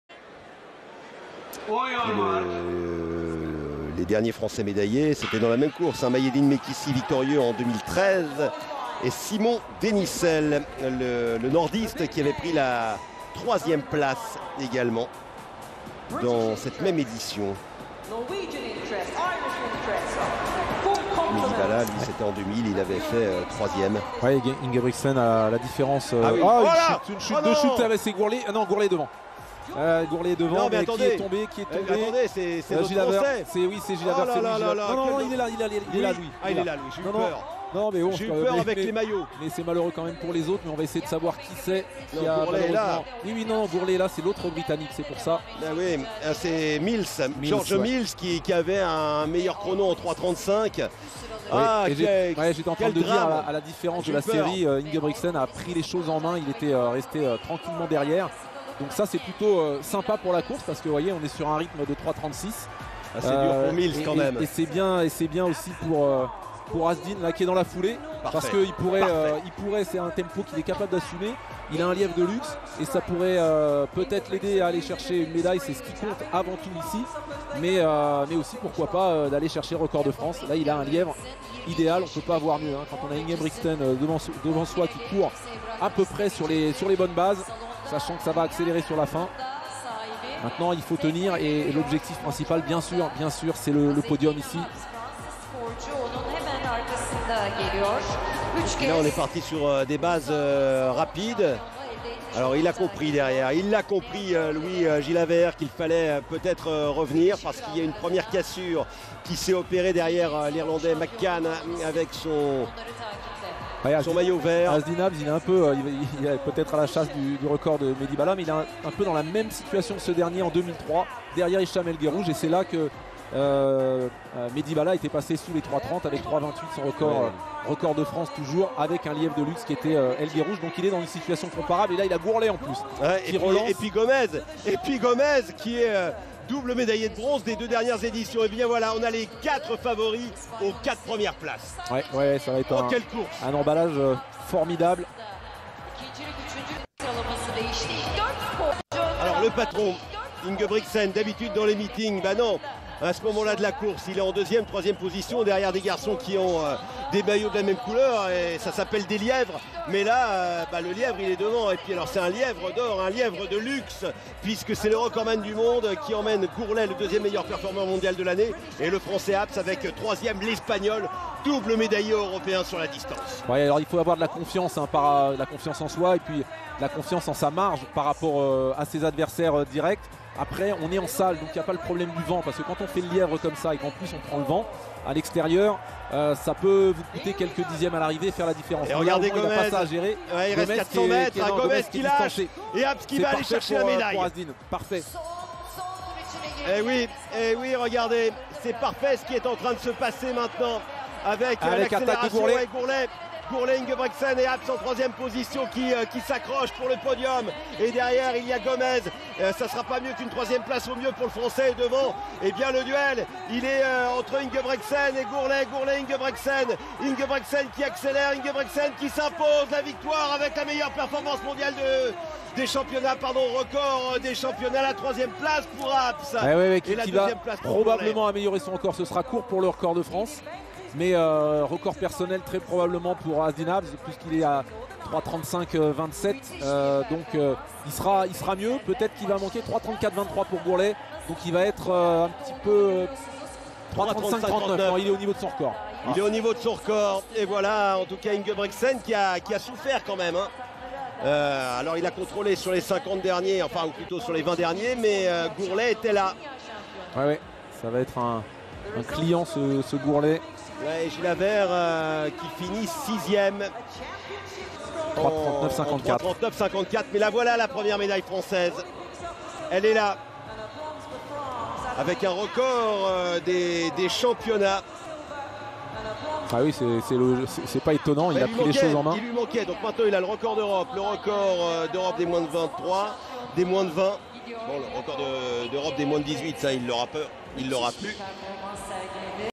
1 500 m Finale Championnats d'Europe en salle 2023 Istanbul Jakob Ingebrigtseni
1500 m Hommes - 3 Mars 2023

1 NORJakob Ingebrigtsen (NOR) 3:33.95
2 ECONeil Gourley (ECO) 3:34.23
3 FRAAzeddine Habz (FRA) 3:35.39
4 ESPJesús Gómez (ESP) 3:38.11
5 ITAPietro Arese (ITA) 3:38.91
6 FRALouis Gilavert (FRA) 3:39.54
7 BELIsmael Debjani (BEL) 3:40.06
8 RTCJan Fris (RTC) 3:40.86
9 POLMichal Rozmys (POL) 3:43.09
10 IRLLuke McCann (IRL) 3:44.55
11 G-BGeorge Mills (G-B) 3:51.28
12 ITAOssama Meslek (ITA) DNF